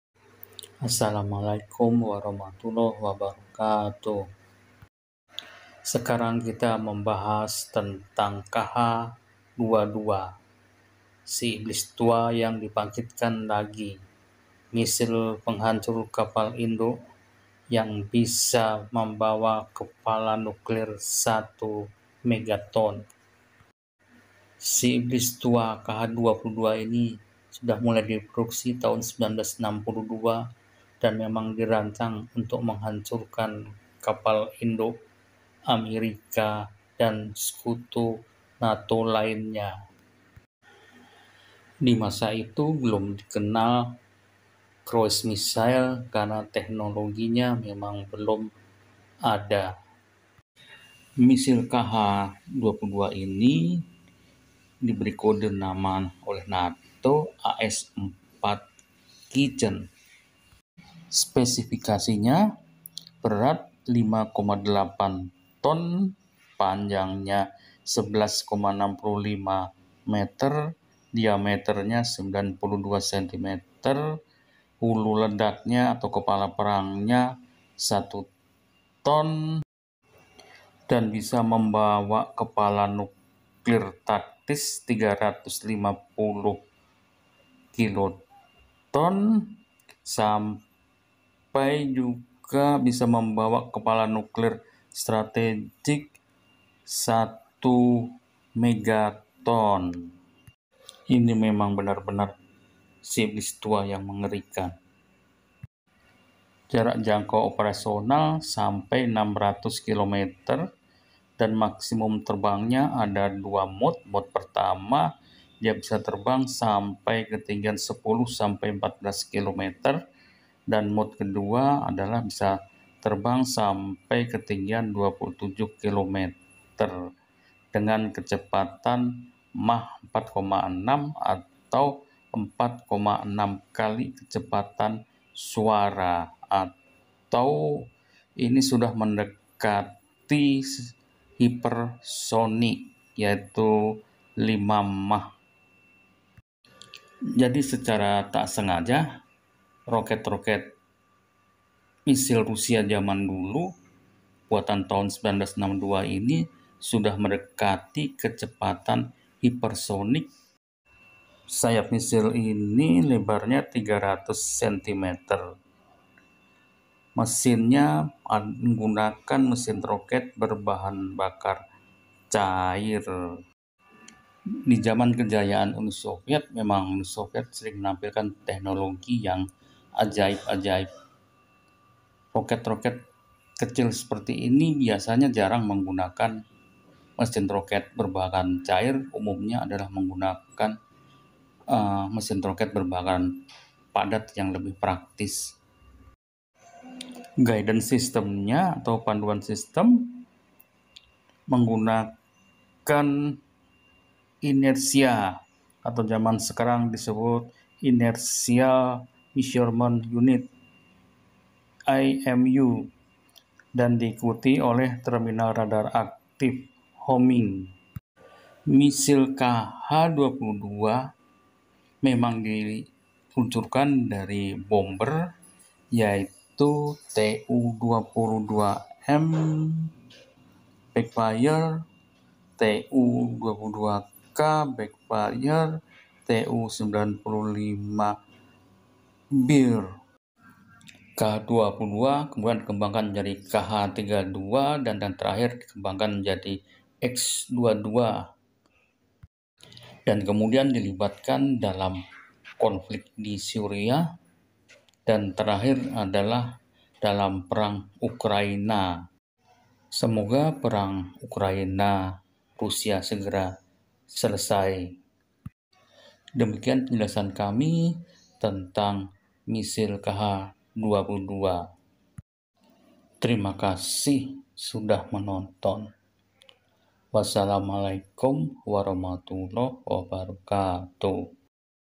Rudal Kh 22 Si Iblis Tua Kec Super Sonik.Dpt Membawa Bom Nuklir Taktis.Digunakan Menyerang Ukraina
KH 22 Si Iblis Tua yg dibangkitkan lagi. Missile Penghancur Kapal Induk

Mulai Diproduksi 1962.

Kh-22 menggunakan mesin roket Isayev bahan bakar cair, berbahan bakar hidrazin dan IRFNA (menghambat asam nitrat red), memberikan kecepatan maksimum Mach 4,6 dan jangkauan hingga 600 km (320 nm). Hal ini dapat diluncurkan baik tinggi-ketinggian atau mode ketinggian rendah. Dalam modus ketinggian tinggi, naik ke ketinggian 27.000 m (89.000 kaki) dan membuat menyelam kecepatan tinggi ke target, dengan kecepatan terminal sekitar Mach 4.6. Dalam modus ketinggian rendah, itu naik ke 12.000 m (39.000 kaki) dan membuat menyelam dangkal di sekitar Mach 3,5, membuat pendekatan akhir pada ketinggian di bawah 500 m (1.600 ft). Rudal dipandu oleh autopilot gyro-stabil dalam hubungannya dengan altimeter radio.

spesifikasi:
Berat 5.820 kg (12.800 pon)
Panjangnya11,65 m (38,2 kaki)Diameternya 92 cm (36 inci)
Hulu ledak1.000 kg (2.205 lb) RDX
atau
350–1.000  kt(1,5–4,2 PJ) Senjata Nuklir
Sayap lebar300 cm (120 inci).

Jangkauan operasional

600 km (320 nmi) (Kh-22M/MA)
Tinggi Terbang 10–14 km (33.000–46.000 kaki) atau 27 km (89.000 kaki)
Kecepatan maksimumMach 4,6 (5,600 km/jam; 3,500 mph)